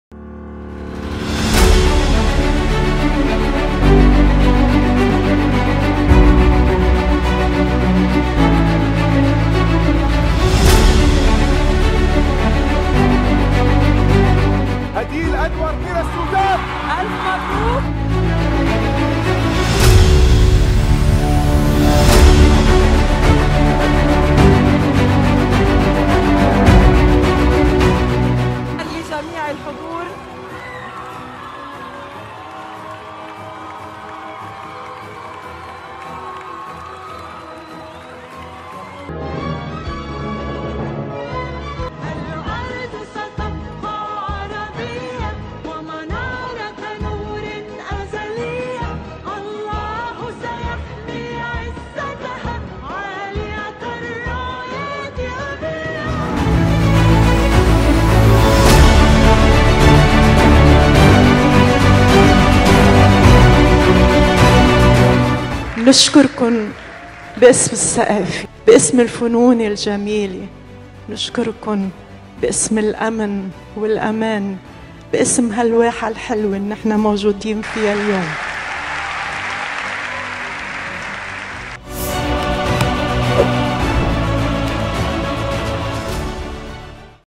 "تحدي القراءة" الإمارات تعيد أمجاد المعرفة العربية
إنجاز جديد، تسجله الإمارات، للمعرفة العربية.. عبر "تحدي القراءة العربي". شاهدوا أجواء التكريم في هذا الفيديو من "البيان".